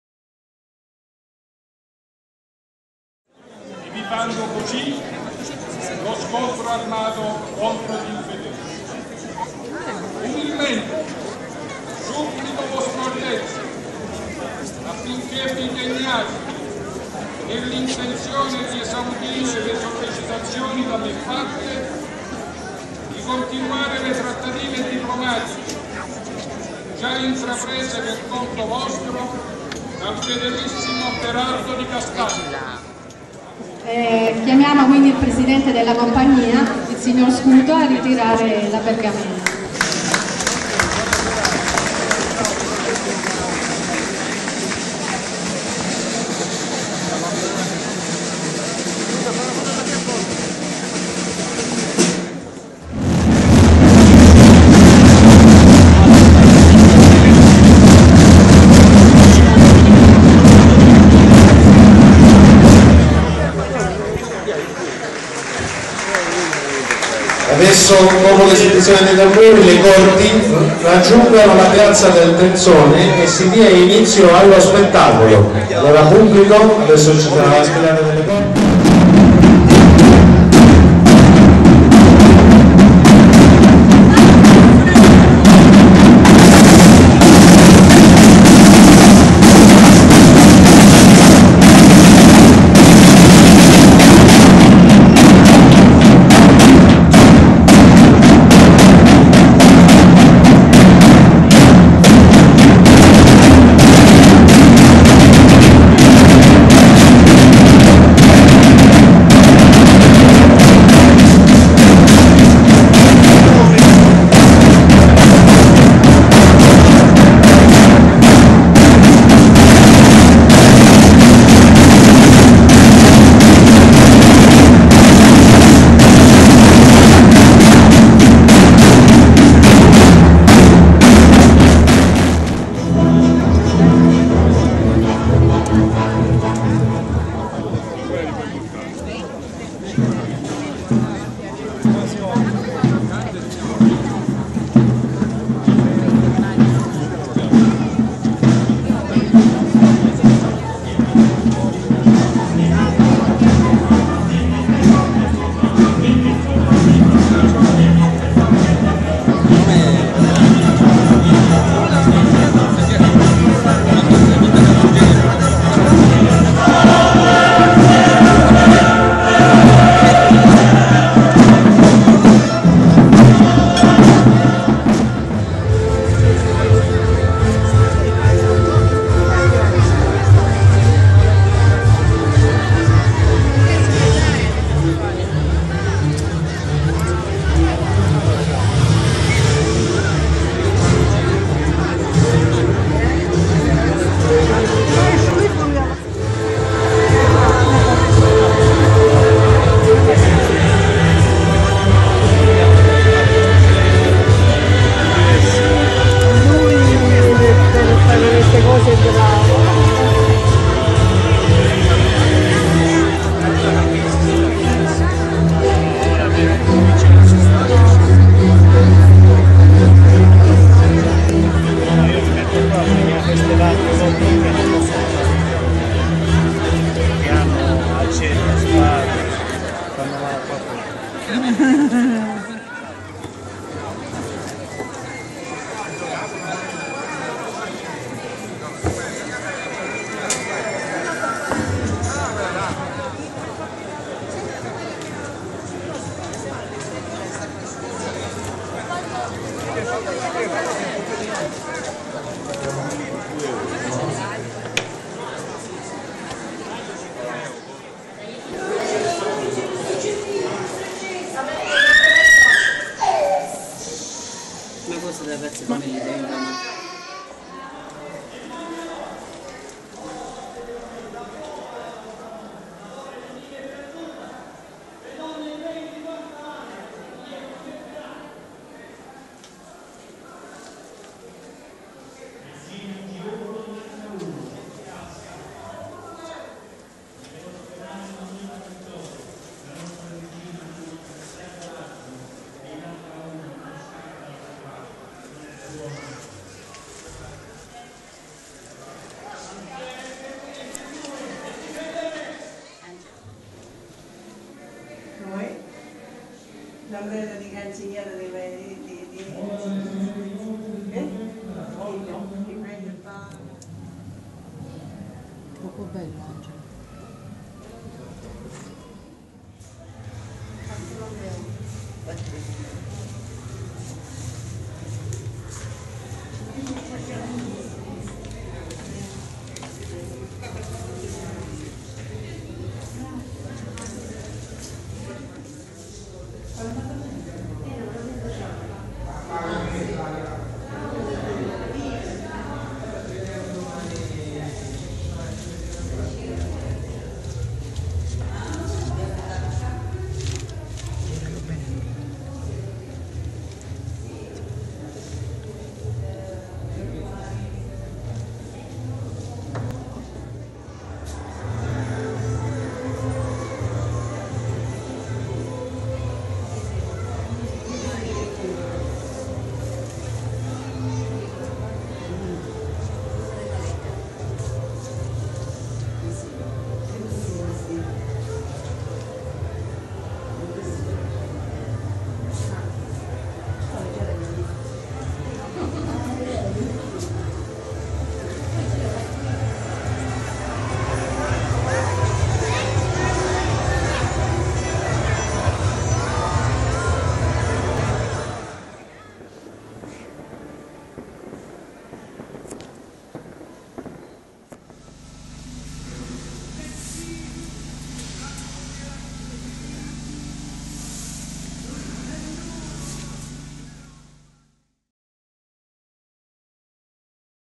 Alla Corte di Federico II° di Svevia a Santa Lucia del Mela (Me) 28/7/2012
Bellissima manifestazione tenutasi nello splendido borgo medievale di  Santa Lucia del Mela ed organizzata dall'Associazione Culturale Antiche Torri, con la degustazione gratuita di prodotti enogastronomici, la mostra mercato, le visite guidate alle chiese ed allo splendido palazzo vescovile, il tutto accompagnato dalla tipica atmosfera medievale con giocolieri, spettacoli del fuoco e musiche a tema. Nel video in questione e' possibile ammirare alcuni momenti del corteo storico di Federico II° di Svevia, della partita a scacchi in piazza con i personaggi dello stesso corteo, ed, infine, gli interni del palazzo vescovile.